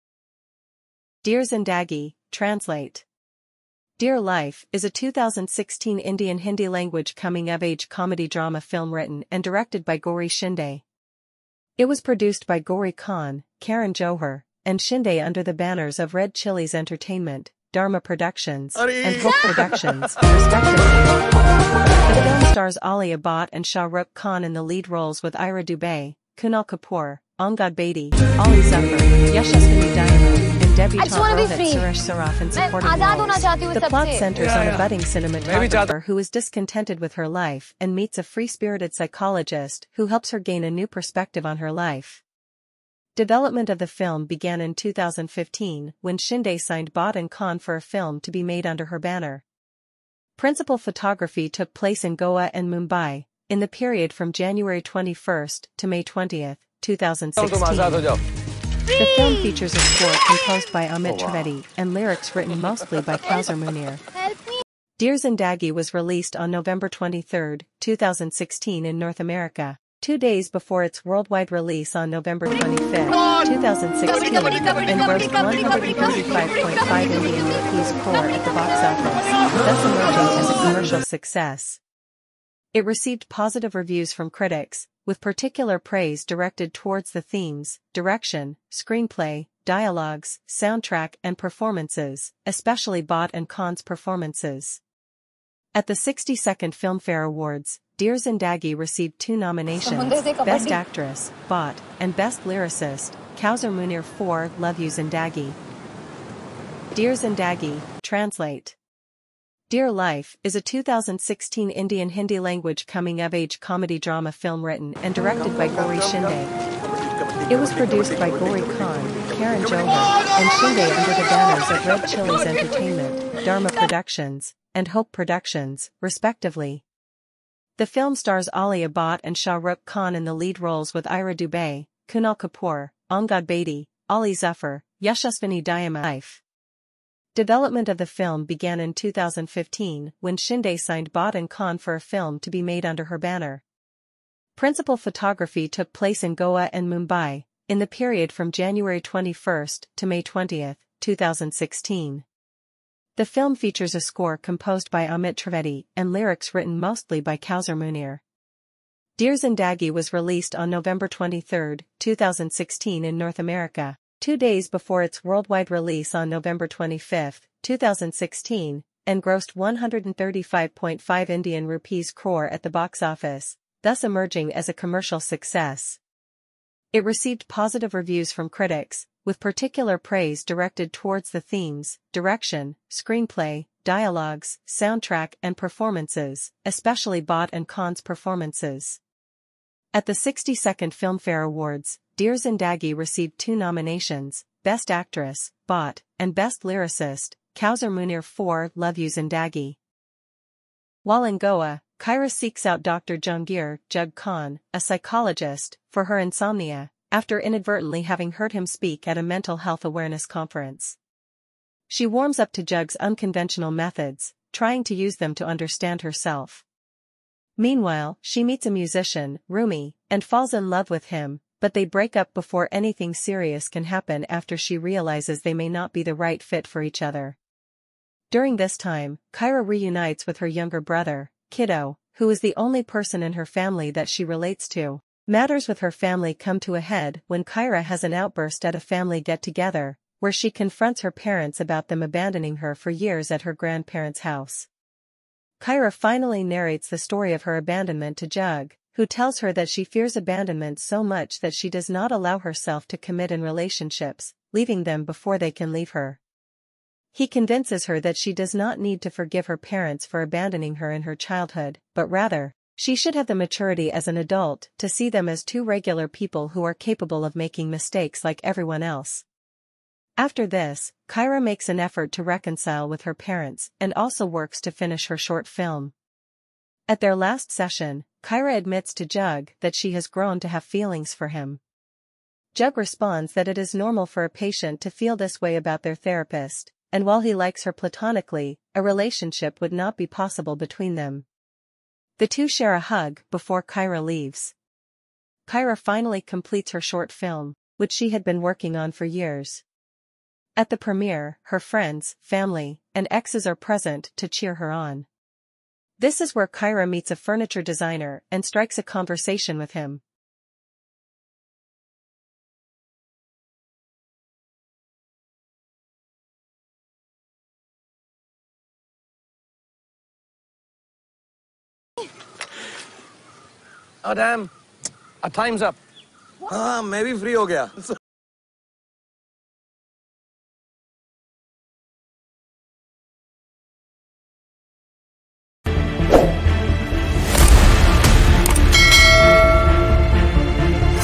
Dear Zindagi Movie Review: A Must-Watch Film
Dive into the world of Indian cinema with our review of 'Dear Zindagi'! Explore the captivating narrative, stellar cast, and poignant themes that make this film a must-watch for audiences worldwide.